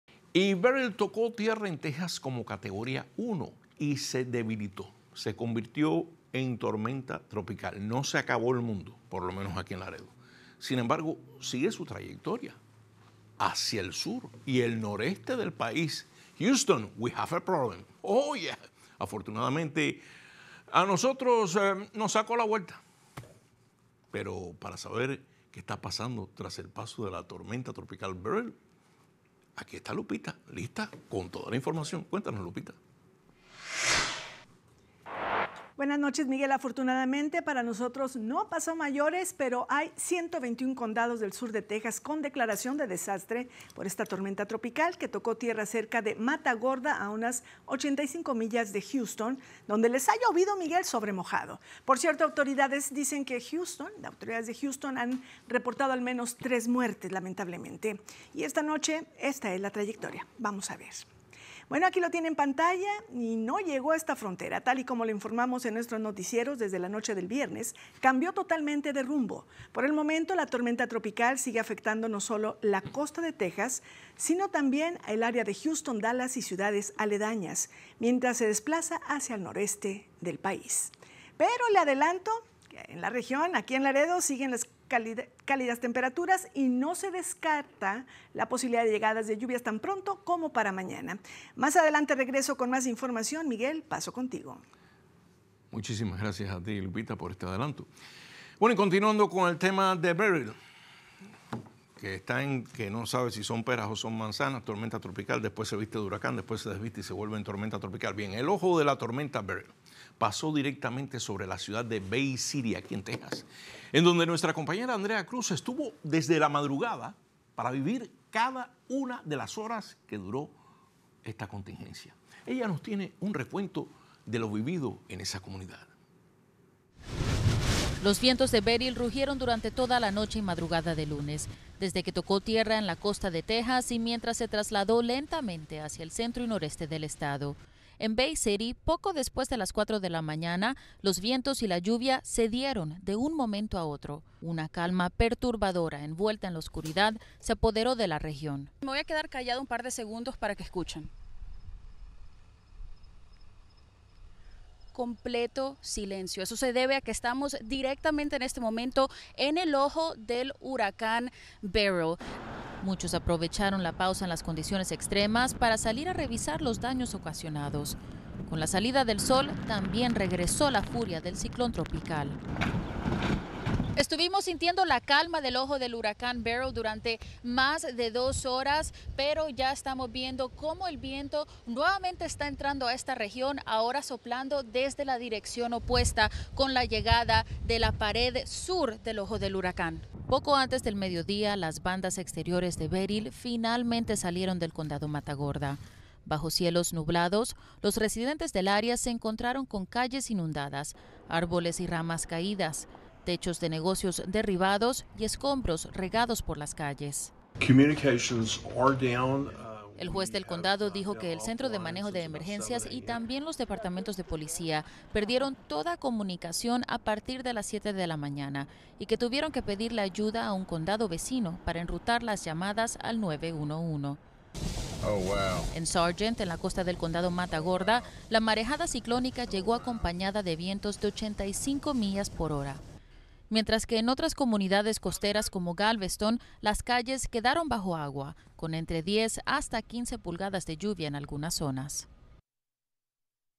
Paso de la tormenta "Beryl" por Texas
Teneoticias KGNS Edición 10:00pm del Lunes 8 de Julio de 2024